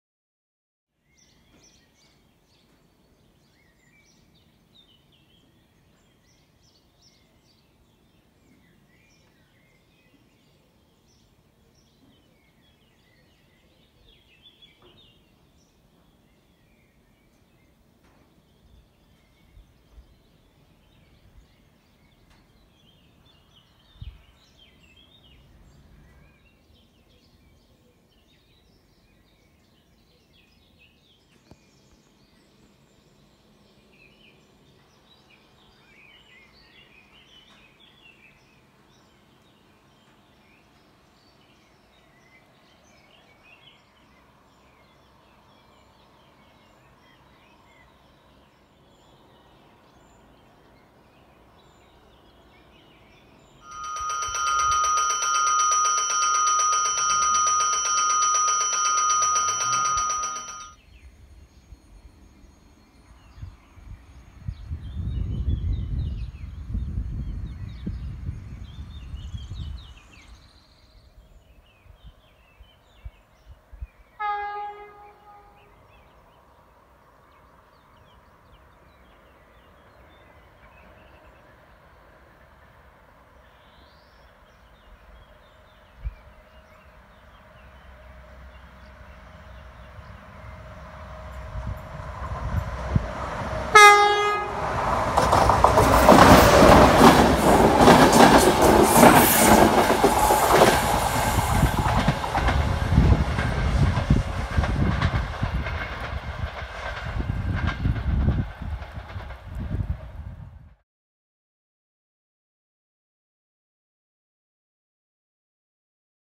E403.005 + 2 vetture con l’INVIO Bari C.le - Roma Termini, in transito ad Ariano Irpino
Ariano Irpino il 26/06/2020